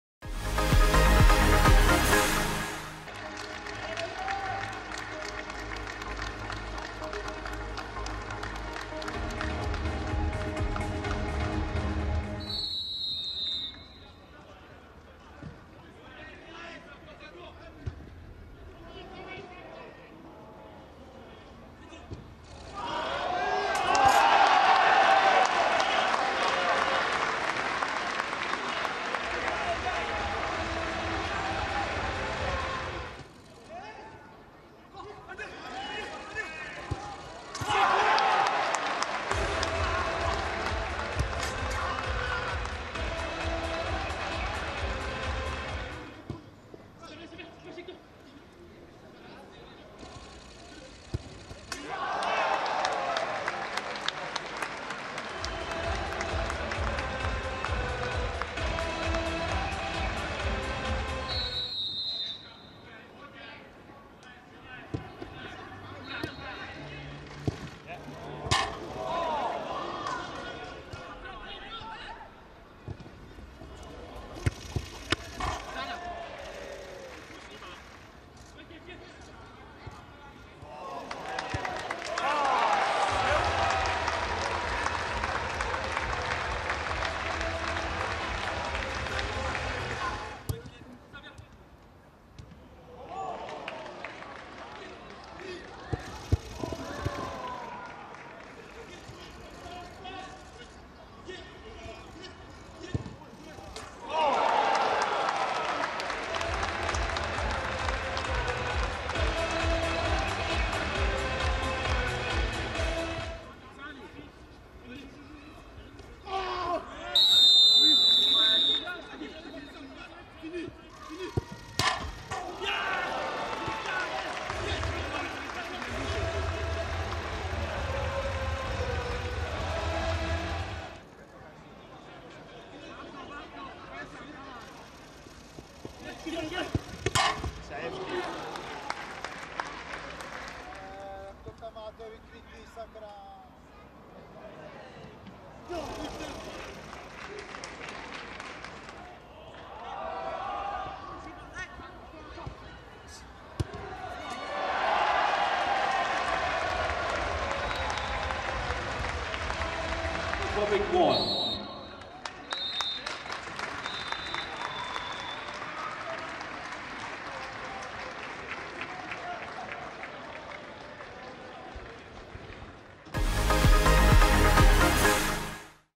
U17 Quarter-final highlights France 6-1 Czech Republic
Watch all the action as France beat Czech Republic in the first quarter-final at the Tallaght Stadium, Dublin